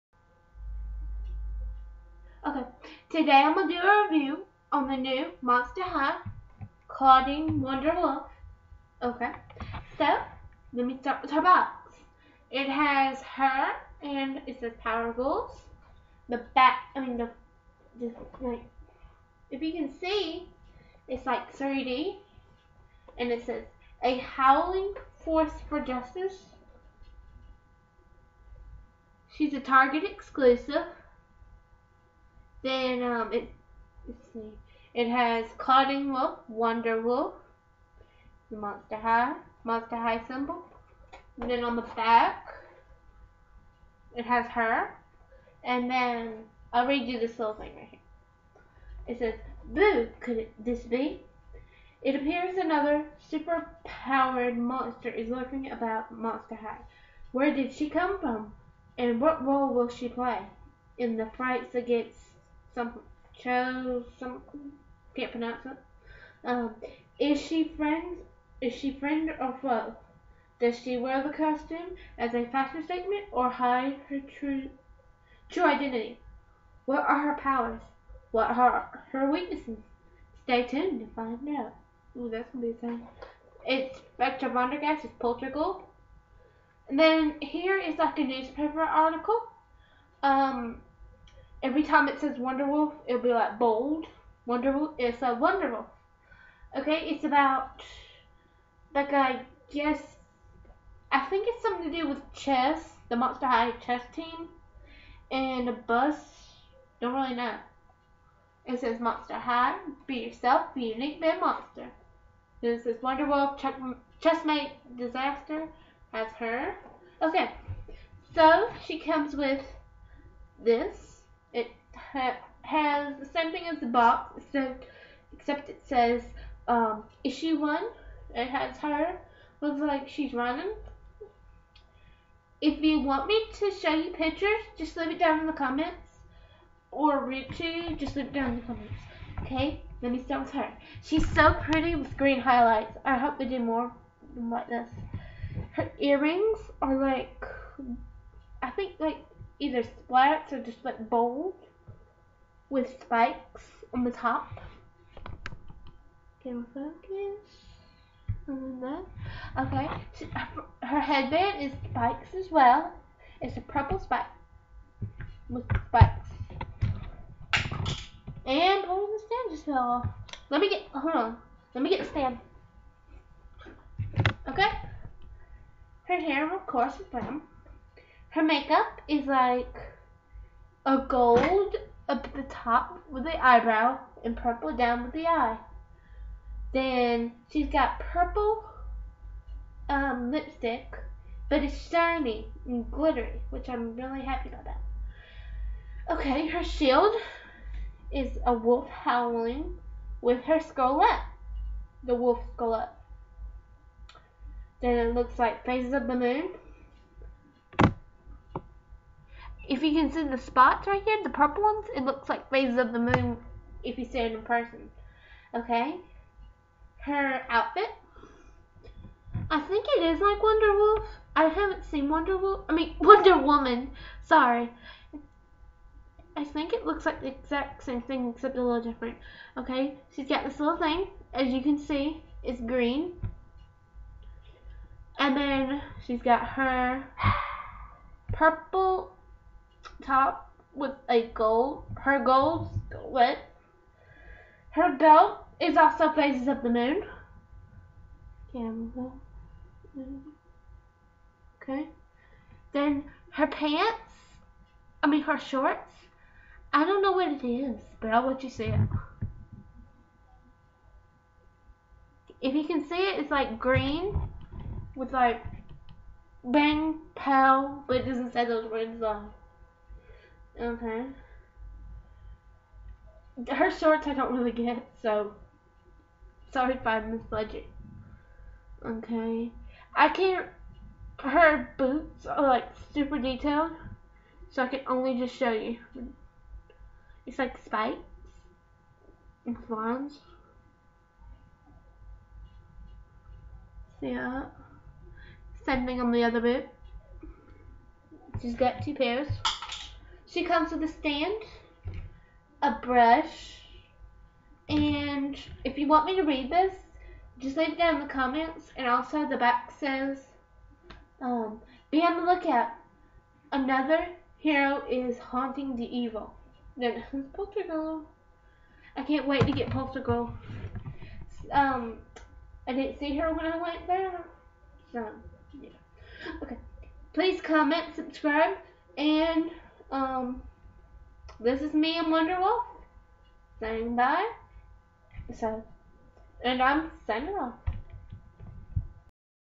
review on wonder wolf!!!!:)
i got wonder wolf at target for 13.98 plus taxs